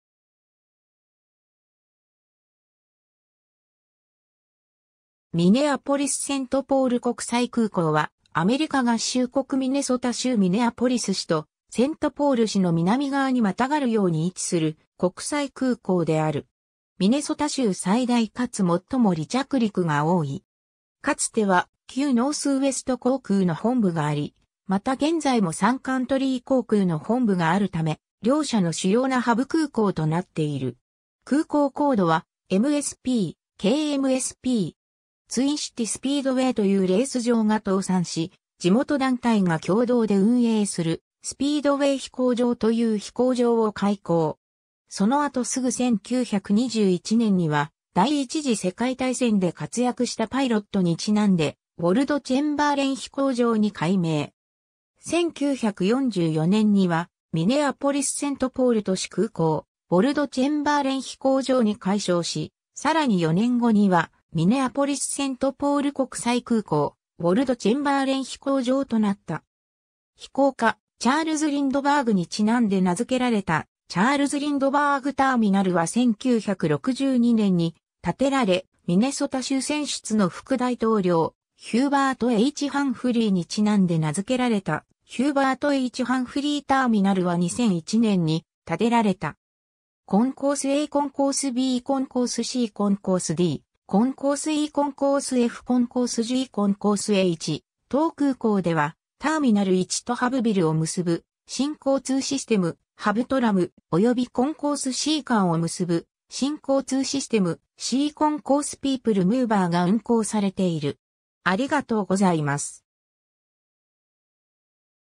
ミネアポリス=セントポール国際空港
ミネアポリス=セントポール
ミネアポリス=セントポール国際空港（ミネアポリス=セントポールこくさいくうこう、英: Minneapolis-Saint Paul International Airport）は、アメリカ合衆国ミネソタ州ミネアポリス市とセントポール市の南側にまたがるように位置する国際空港である。
 ミネソタ州最大かつ最も離着陸が多い。
かつては旧ノースウエスト航空（現デルタ航空）の本部があり、また現在もサンカントリー航空(Sun Country Airlines)の本部があるため、両社の主要なハブ空港となっている。
空港コードはMSP(IATA)/KMSP(ICAO)。
 ツインシティ・スピードウェイというレース場が倒産し、地元団体が共同で運営する「スピードウェイ飛行場（Speedway Field）」という飛行場を開港。
その後すぐ1921年には、第一次世界大戦で活躍したパイロットにちなんで、ウォルド・チェンバーレン飛行場に改名。
1944年には、「ミネアポリス=セントポール都市空港・ウォルド・チェンバーレン飛行場」に改称し、さらに4年後には「ミネアポリス=セントポール国際空港・ウォルド・チェンバーレン飛行場」となった。
飛行家チャールズ・リンドバーグにちなんで名付けられたチャールズ・リンドバーグ・ターミナル（現ターミナル1）は1962年に建てられ、ミネソタ州選出の副大統領ヒューバート・H・ハンフリーにちなんで名付けられたヒューバート・H・ハンフリー・ターミナル（現ターミナル2）は2001年に建てられた。
 コンコースA コンコースB コンコースC コンコースD コンコースE コンコースF コンコースG コンコースH 当空港では、ターミナル1（旧・リンドバーグ・ターミナル）とハブ・ビルを結ぶ新交通システム「ハブ・トラム」、およびコンコースC間を結ぶ新交通システム「Cコンコース・ピープル・ムーバー」が運行されている。